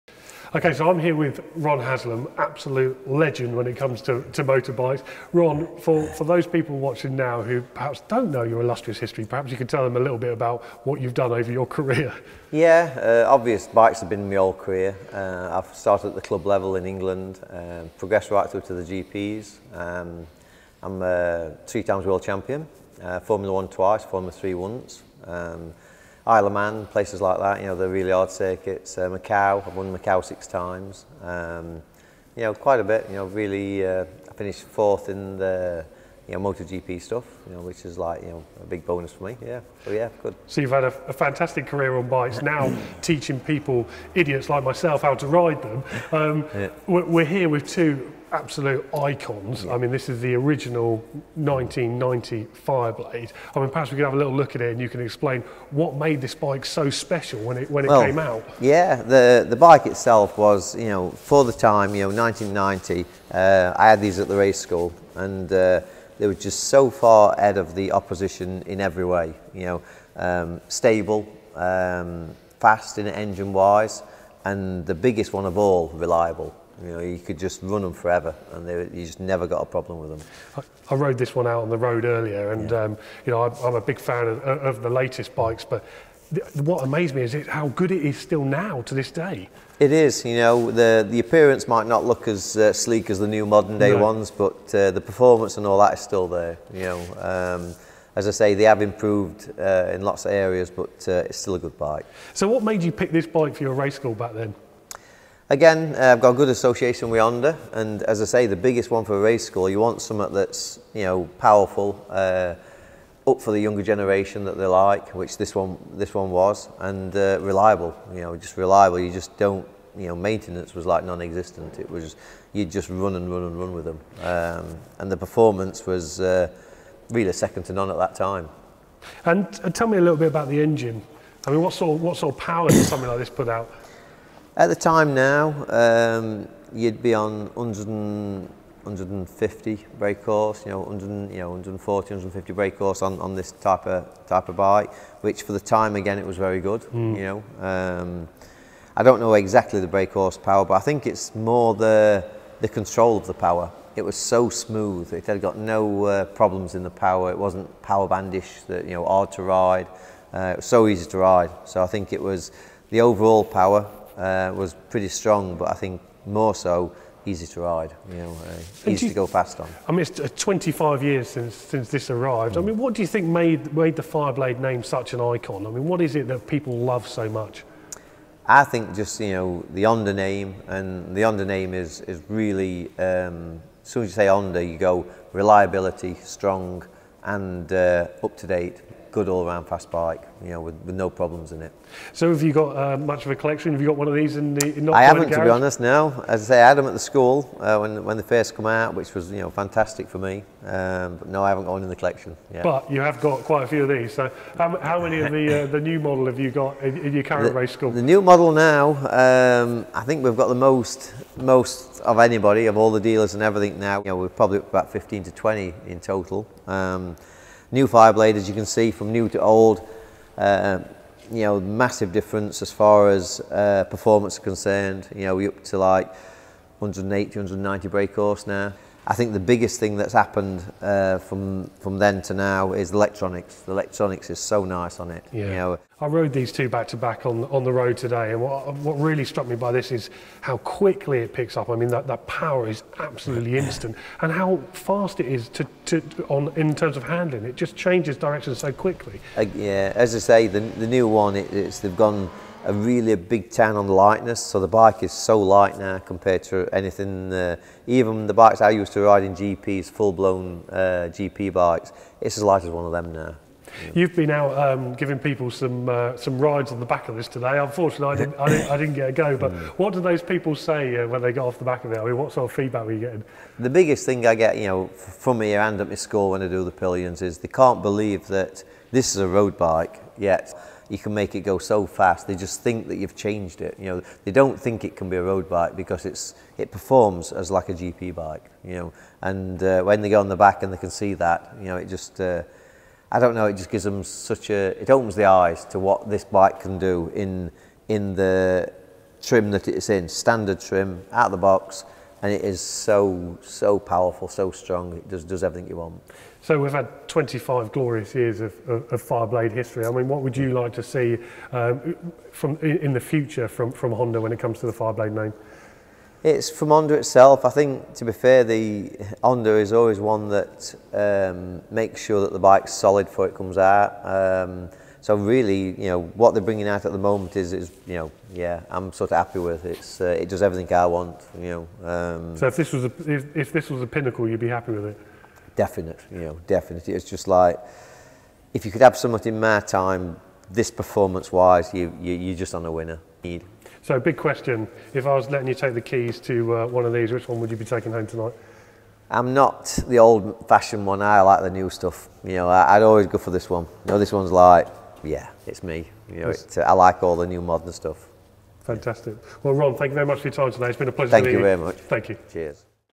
Ron Haslam Talks 25 Years of Honda Fireblade
We chat with motorcycle racing legend Ron Haslam about the new Honda Fireblade and its 25 year legacy